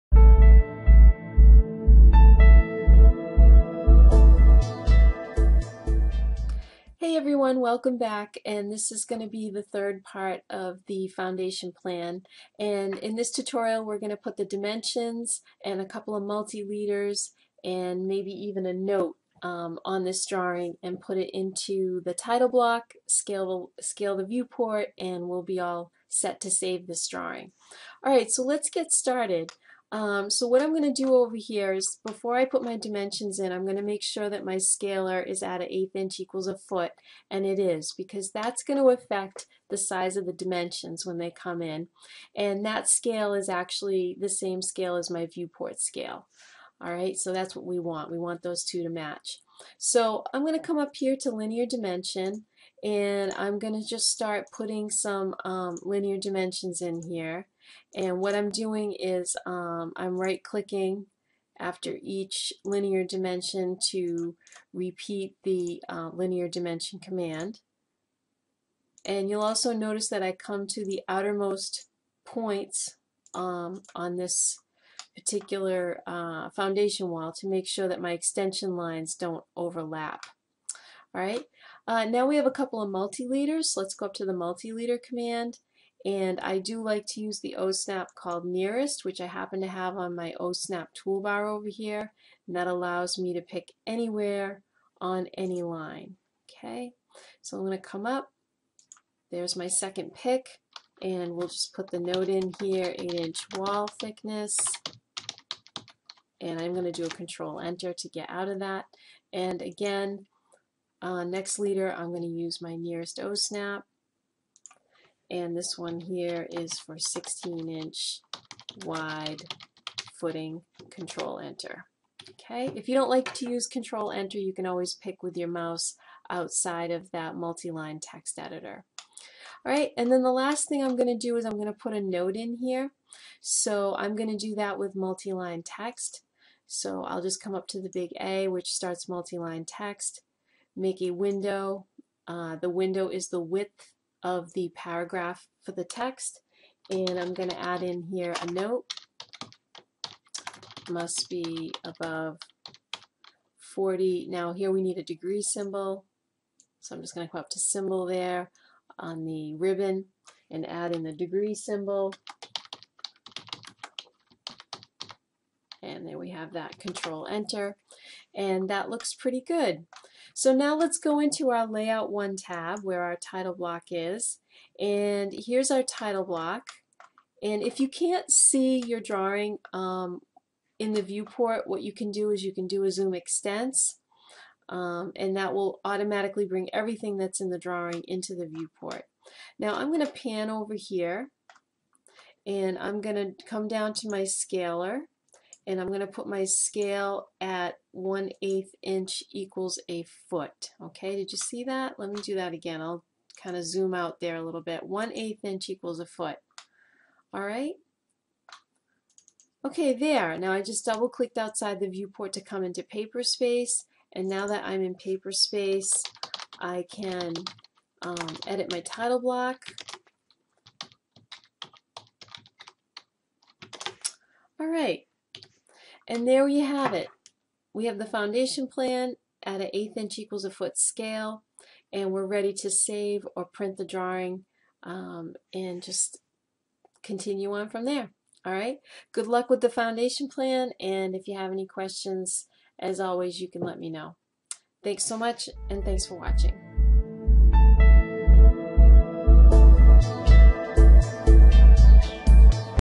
Autocad - Dimensions & Viewport Scale on Foundation Plan Part 3 #38
Add dimensions, leaders and text to the foundation plan.  Scale the drawing in the viewport and edit the titleblock.